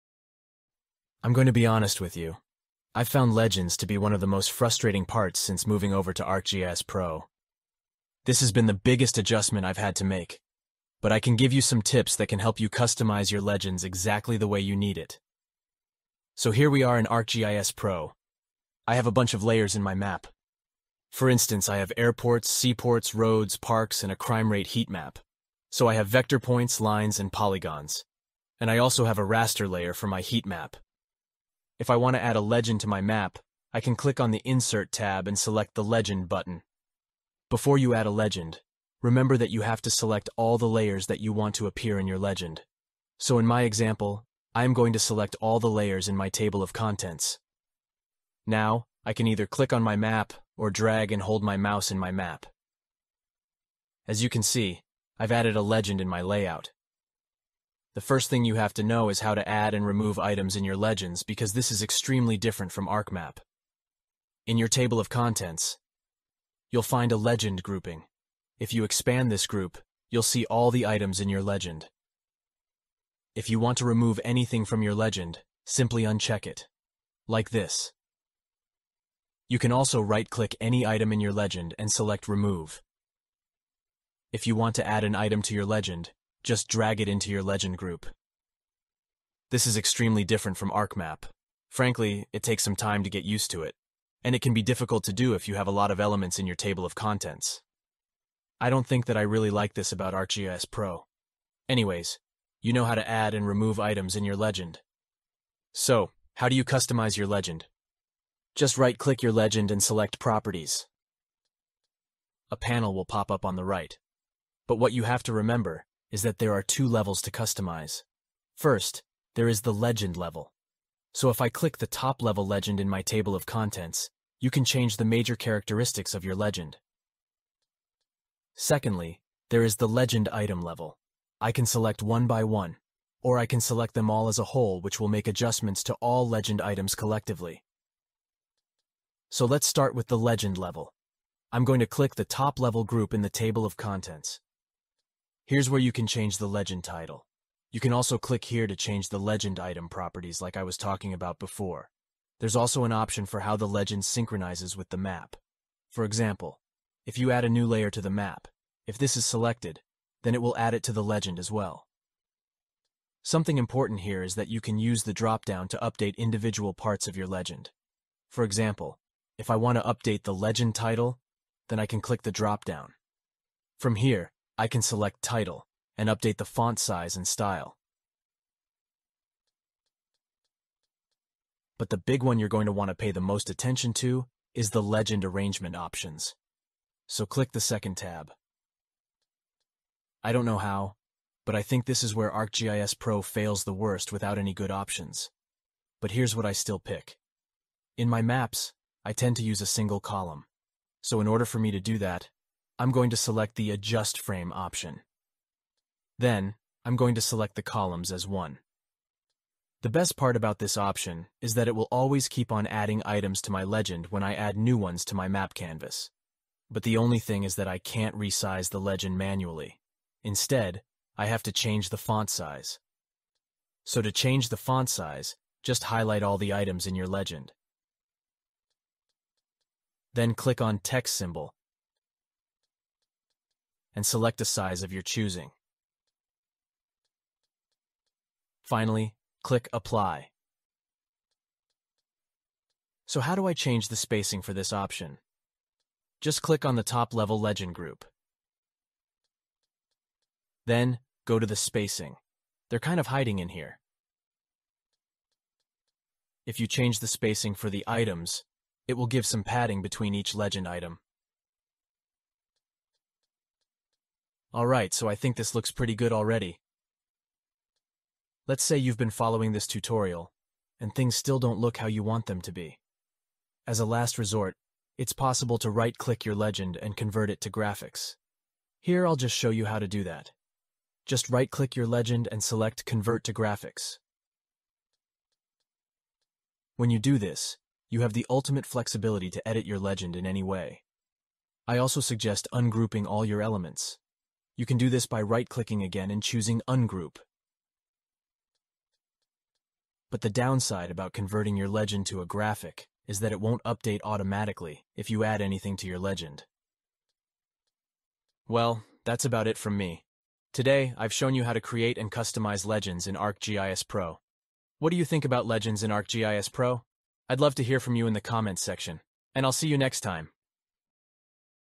Legends Tutorial in ArcGIS Pro
In this video, I'll teach you how to create and customize legends in ArcGIS Pro. You'll learn about legend levels, adding items, and various other aspects.  I'll also teach you how to convert legends to graphics and ungroup items for ultimate flexibility.